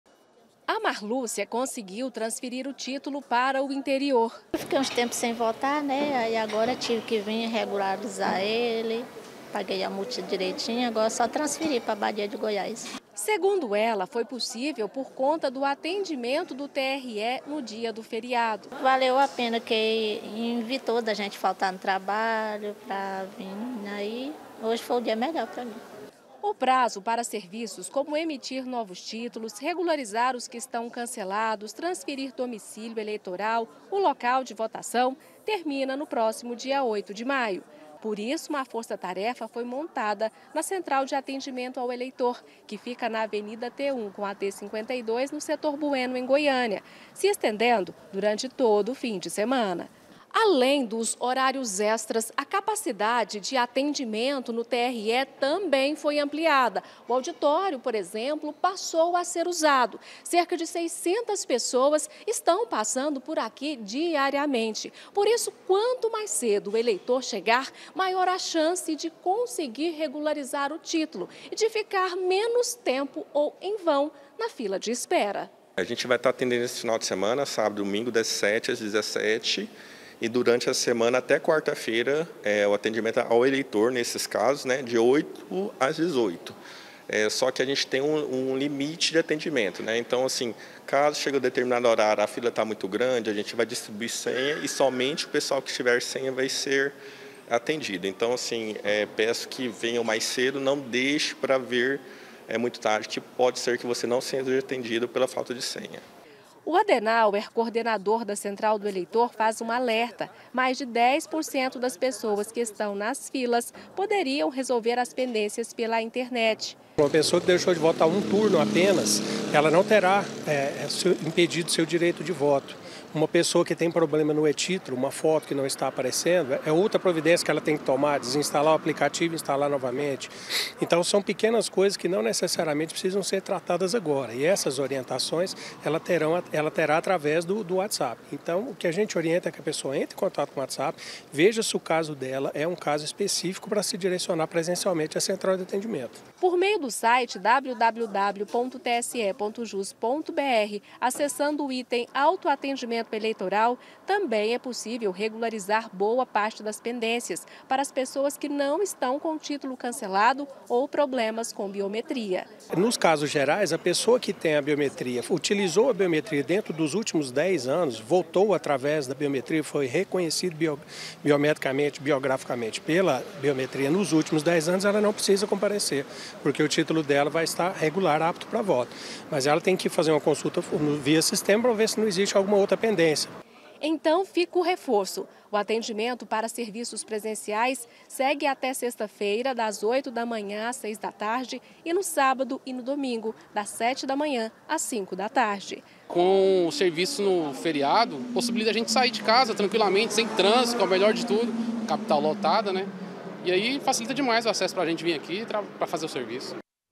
ELEIÇÕES 2024: TRE MONTA FORÇA-TAREFA PARA ATENDER ELEITORES
O PRAZO PARA REGULARIZAR O TÍTULO DE ELEITOR TERMINA DAQUI UMA SEMANA, DIA 8 DE MAIO. POR ISSO, MUITA GENTE APROVEITOU O FERIADO PARA IR ATÉ O TRE PARA RESOLVER PENDÊNCIAS COM A JUSTIÇA ELEITORAL.